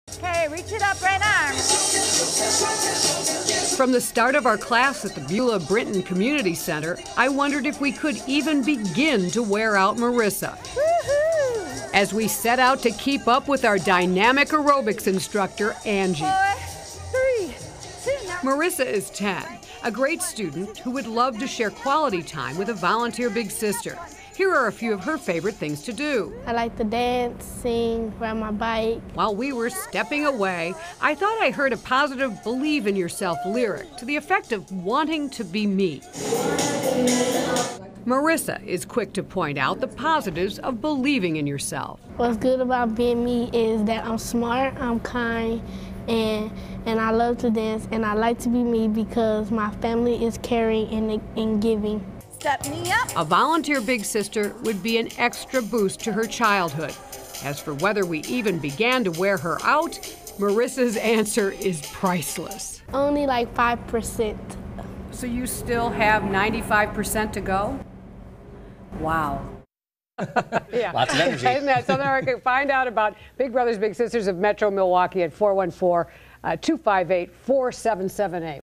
Tuesday's Child Marissa enjoys singing, dancing
Marissa, 10, a great student who would like to spend quality time with a volunteer Big Sister.